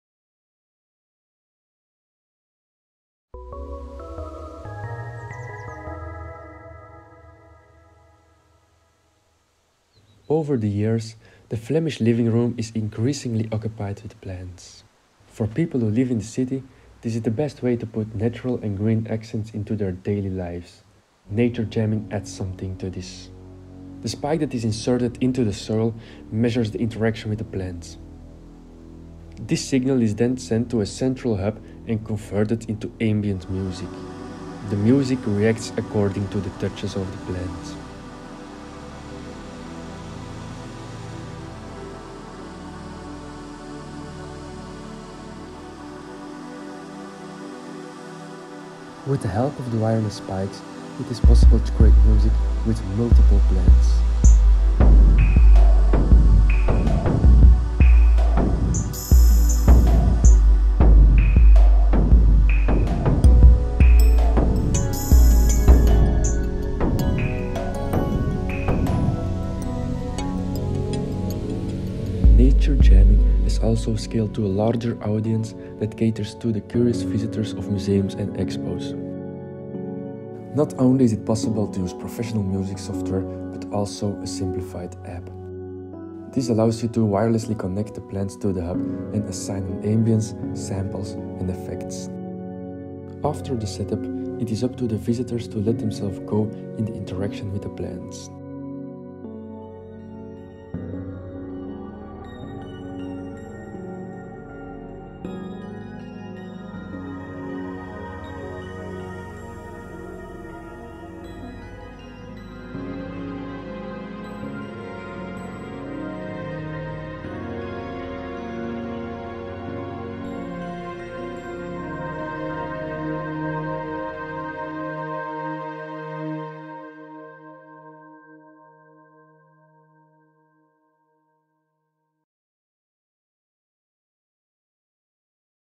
2021 - Case 6 - Nature Jamm'n: Nature's touch on serendipity
Nature's touch on serendipity

Programming Nature jamm'n is a project developed by four Industrial Engineering design students at Ghent University. With the green project they want to take the interaction of man and nature to a higher musical level. Over the years, the Flemish living room has become increasingly occupied with plants. For people living in the city, this is the best way to set natural and green accents. Nature Jamm'n adds something to this. With the help of a user-friendly product you are able to make music by touching the plant. Every music fanatic can enjoy a completely new experience in developing music and sound.
Nature Jamm'n has also scaled to a larger audience that focuses on curious visitors to museums and exhibitions. It brings nature into an urban environment on a level never experienced before. This allows man to rediscover his own senses. Finally, the public aspect gives way to a new way of connecting strangers and creating conversations.